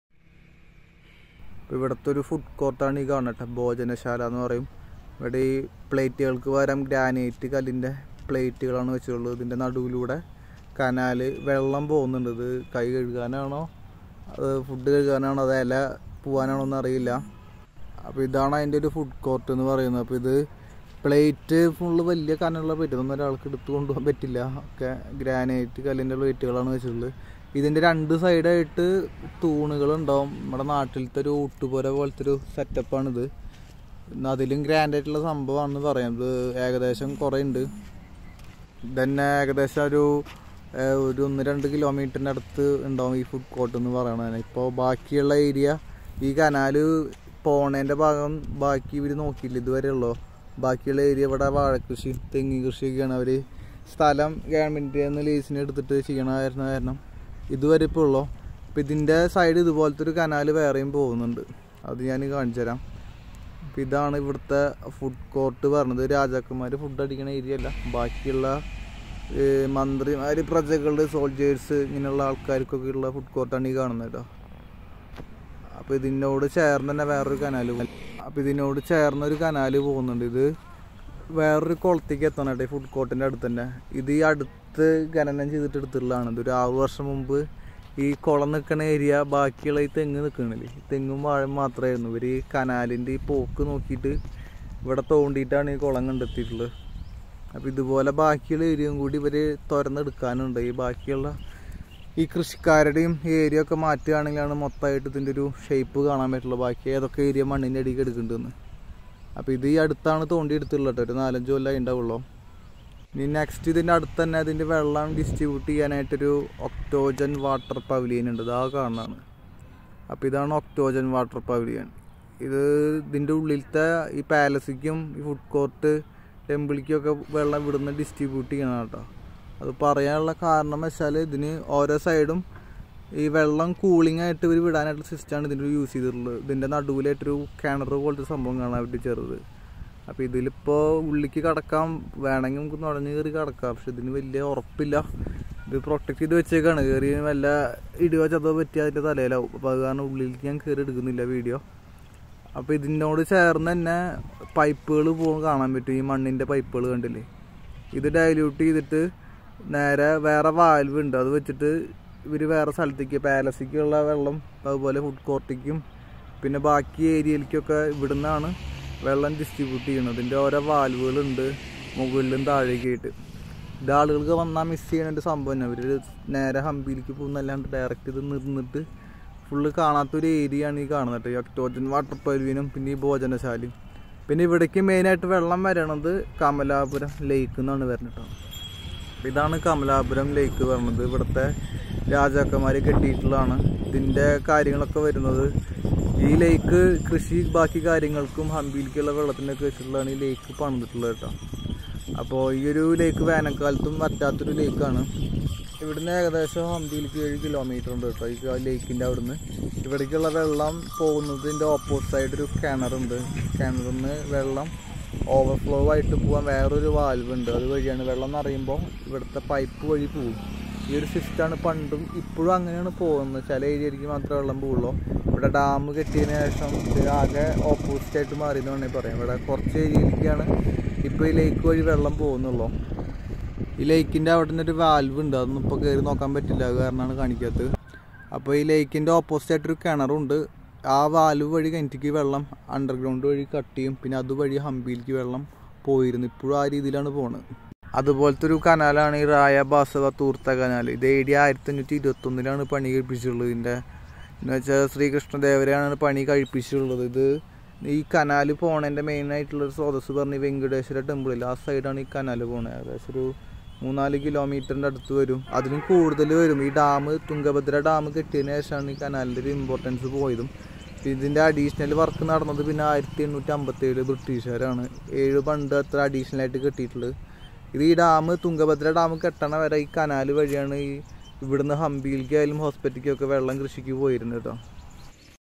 Hampi Hidden Places Part 5 Ancient Dining Bhojana Shala Octagonal Water Pavilion Kamalapura Lake
The Octagonal shaped tent near the main roadside could have been once a spectacular water fountain. This is now practically in an abandoned stage with bushes and wild grass growing around it. You can see this protruding into the main road on the right side of the Kamalapura- Hampi main road, somewhere close to the Chandrashekara temple

If you stop and take a close look, you can see the remains of the water ducts, fountain parts etc. Just across the road is the Bhojanasala, the community feeding area with rows of food plates carved on the rock slabs.